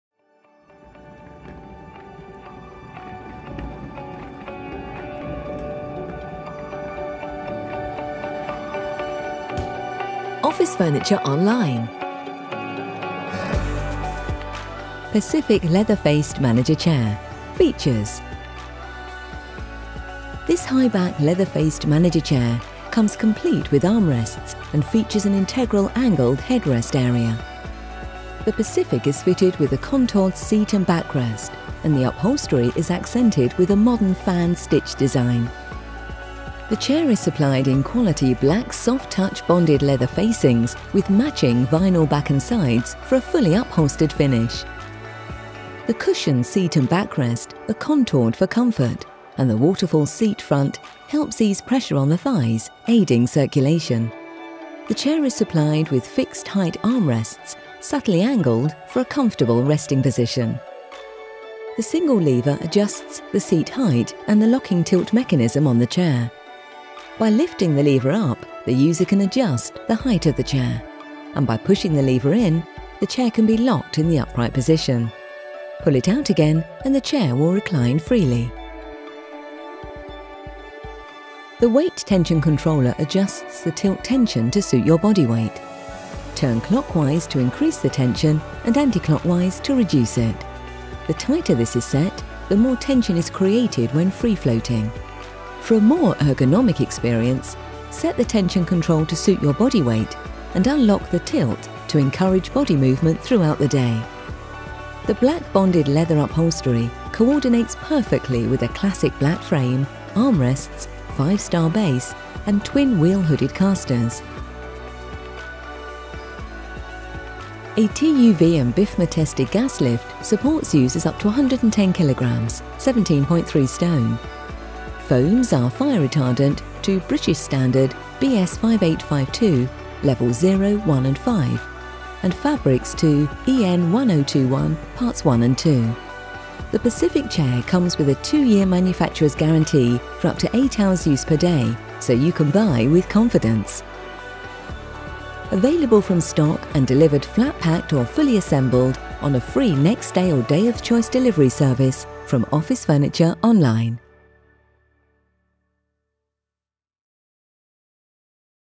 Pacific Leather Faced Manager Chair Features and User Guide
Here's a quick video showing the functions and features of the Pacific Leather Faced Manager Chair  from OfficeFurnitureOnline.co.uk.

The Pacific is available from stock in the UK with Free Next Day or Day of Choice Delivery as flat packed or delivered fully assembled.